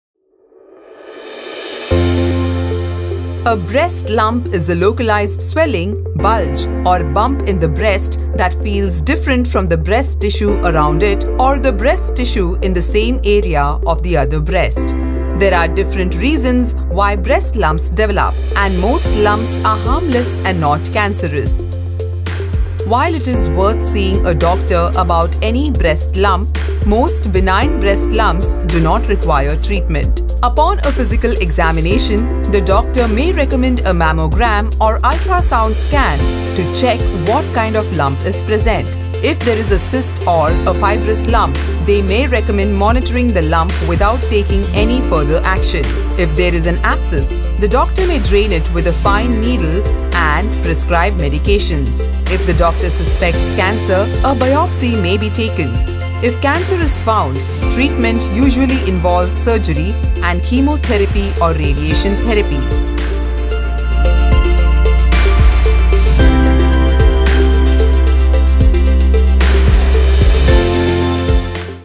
Breast lump | Manipal Hospitals Bengaluru
A breast lump is a swelling, bulge, or bump that develops in the breast for a variety of reasons, and it is mostly benign and not cancerous. Although it is better to consult a doctor about it; however, most benign breast lumps require no treatment. The doctor might advise a mammogram or ultrasound scan to identify the lump present. If a cyst or fibrous growth is present, they might suggest monitoring the lump without further treatment. In case of an abscess, the doctor may use a fine needle to drain it and prescribe medications. The doctor might suggest a biopsy if they suspect it to be cancerous.